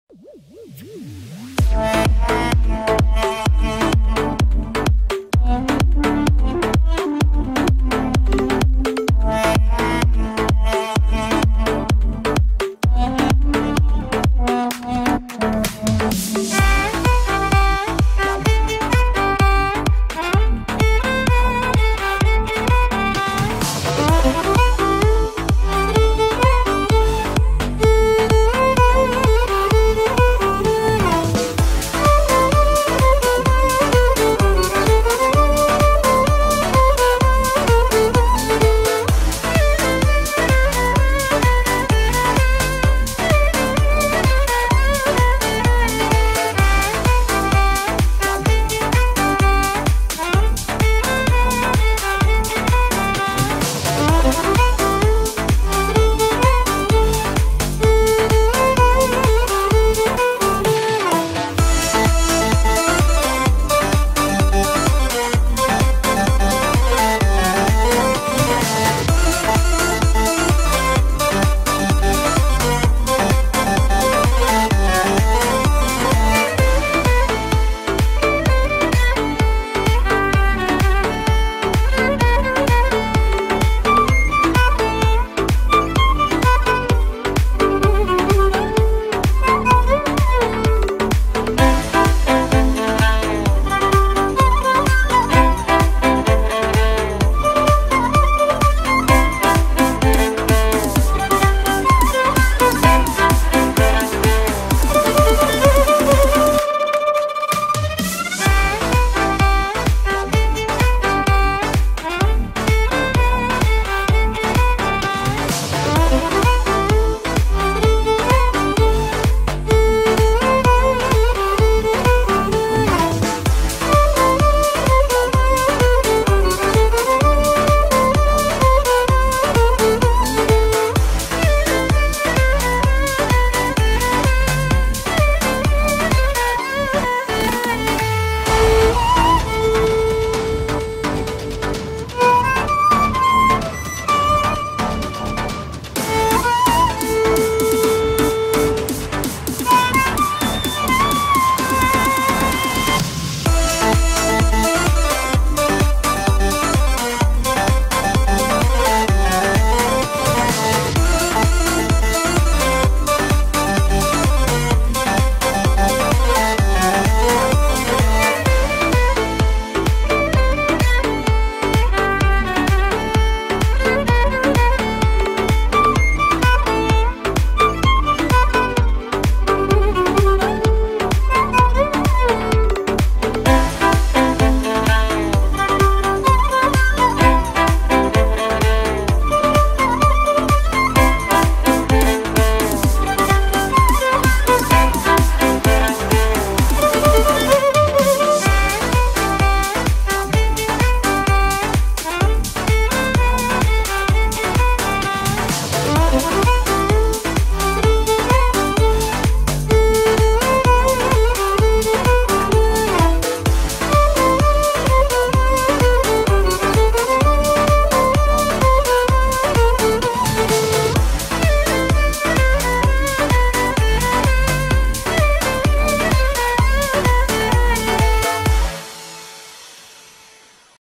Махмадулло и Абдулло, Две скрипки, лето 2023 2124
Выступают Махмадуллои Рахматулло и Абдулло Толибов, студенты Таджикской государственной консерватории. Наш сайт Абдулло